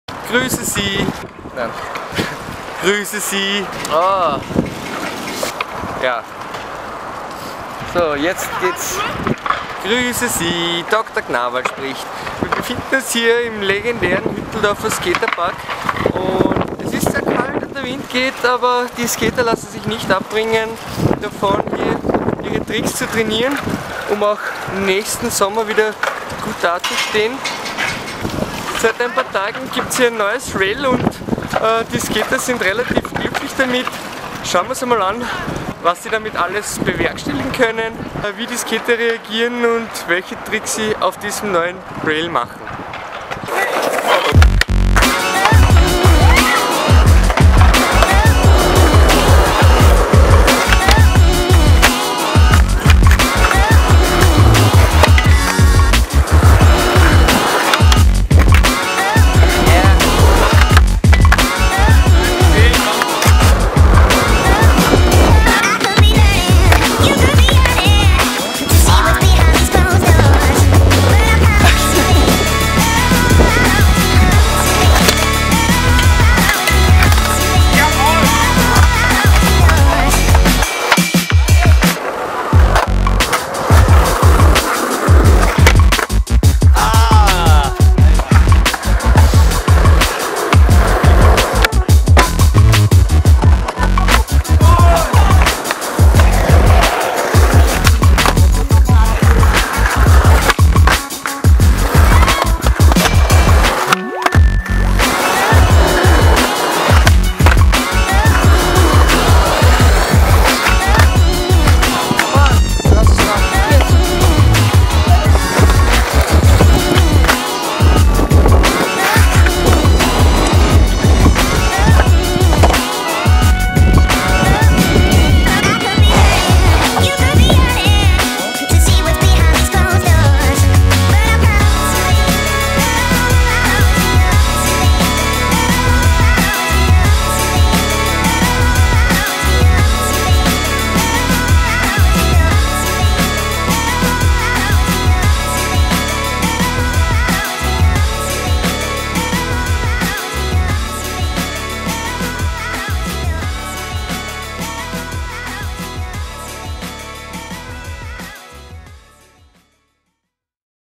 Neuer Flatbar in Hütteldorf - Sloppy Skateboarding - Wien Hütteldorf
Ein paar ortsansässige Skateboarder entdecken einen neues Rail in ihrem Local Skatepark in Wien Hütteldorf. Sie zögern nicht lange und machen ein paar Tricks mit ihren Brettern.